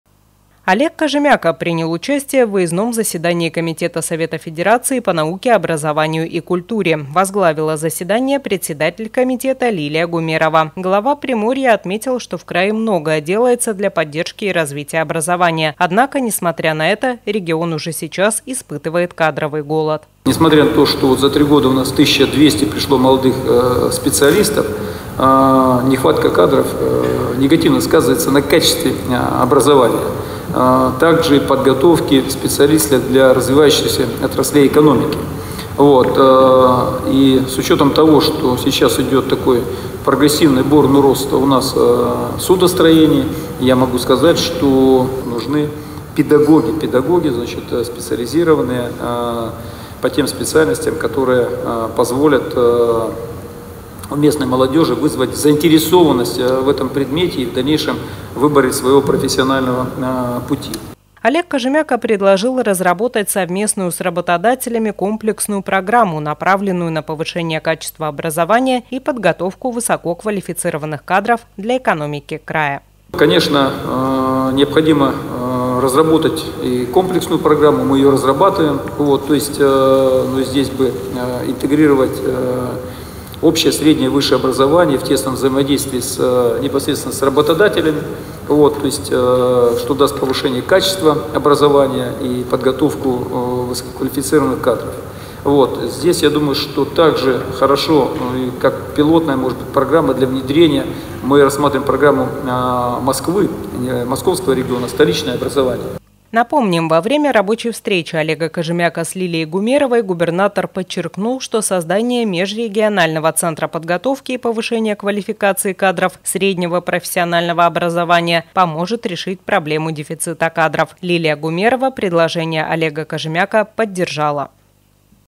Выездное заседание Комитета Совета Федерации.
Олег Кожемяко принял участие в выездном заседании Комитета Совета Федерации по науке, образованию и культуре. Возглавила заседание председатель Комитета Лилия Гумерова. Глава Приморья отметил, что в крае многое делается для поддержки и развития образования. Однако, несмотря на это, регион уже сейчас испытывает кадровый голод.